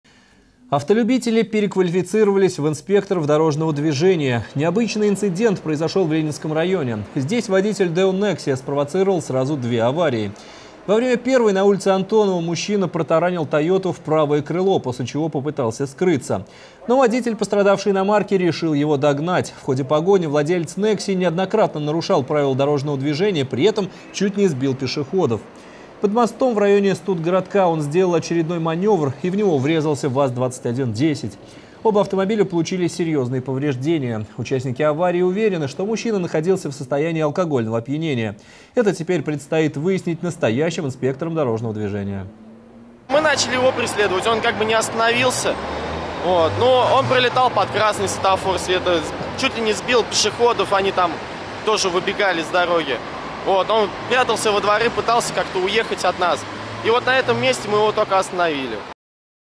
Автолюбители переквалифицировались в инспекторов дорожного движения
Необычный инцедент произошел в Ленинском районе. Здесь водитель спровоцировал сразу две аварии. Во время первой, на улице Антонова, мужчина протаранил авто в правое крыло, после чего попытался скрыться. Подробности - в материале от 9 июля 2015 года.